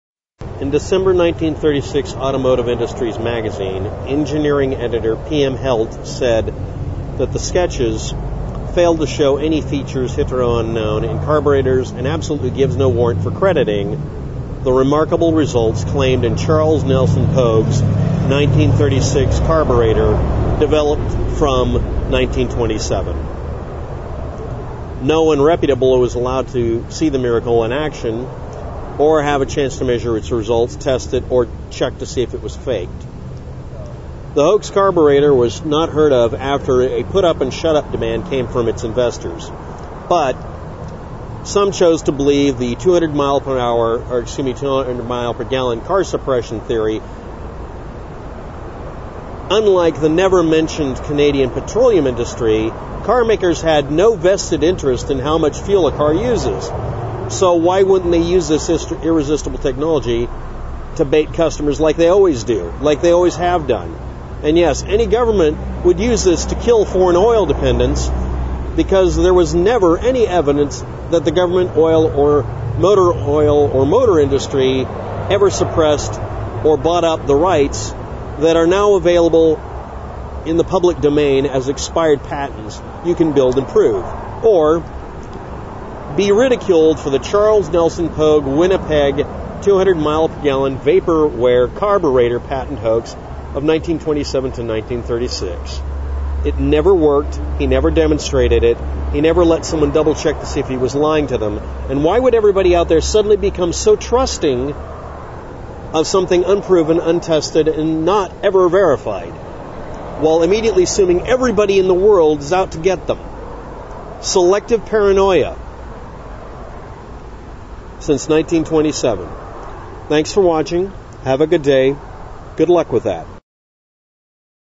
Charles Nelson Pogue Winnipeg 200 MPG Vapor ware Carburetor Patent HOAX 1927 1936 debunk by waptek
[REDACTED]